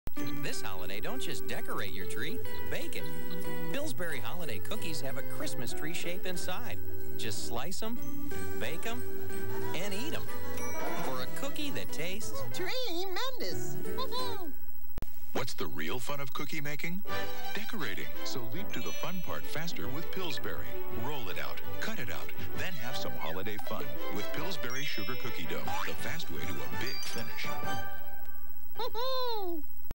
Pillsbury Dough Boy Sugar Cookie Holiday Ads (1996)
Ads for slice and bake and roll out cookie dough